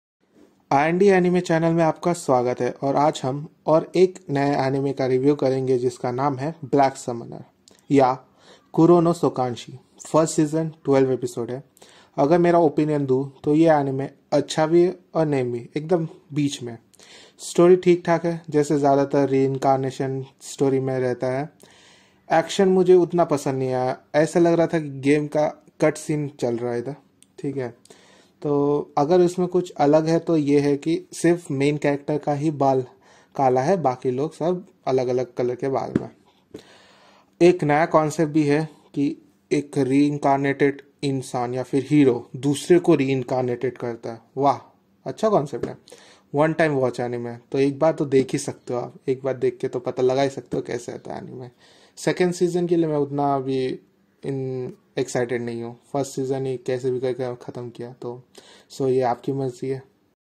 BLACK SUMMONER (REVIEW IN HINDI)
Hello guys welcome to my channel aaj me ek minute me ek aur anime ke bare ,me discuss kar raha hou jiska nam hai black summoner . aur ek naya reincarnated anime hai thik thak hai utna bhi acha nhi hai.